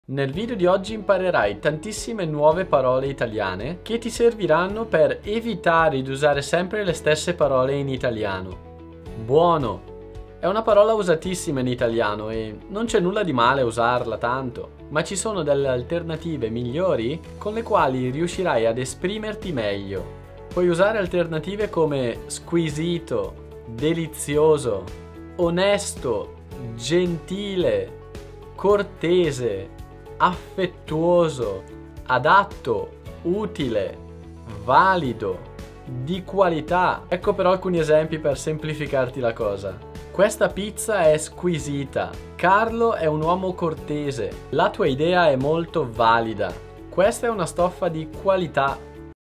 💡ALTERNATIVE VALIDE ALLA PAROLA “BUONO” 🇮🇹
Ciao ragazzi! 🇮🇹 Oggi vediamo insieme diverse alternative SUPER valide da usare al posto di BUONO.

✅ SQUISITO
✅ DELIZIOSO
✅ ONESTO
✅ GENTILE
✅ CORTESE
✅ AFFETTUOSO
✅ ADATTO
✅ UTILE
✅ VALIDO
✅ DI QUALITÀ

Ecco alcuni esempi:
- Questa pizza è squisita.
- Carlo è un uomo cortese.
- La tua idea è molto valida.
- Questa stoffa è di qualità.

E tu usi queste alternative? 🧐